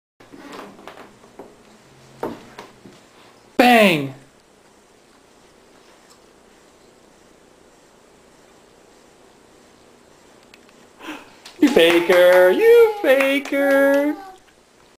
Best dog trick EVER Play Dead with dramatic pug stumble
One video obout Best dog trick EVER - Play Dead with dramatic pug stumble like Husky Dog Talking - " I love you " and Mishka says 12 words - Dog Talking.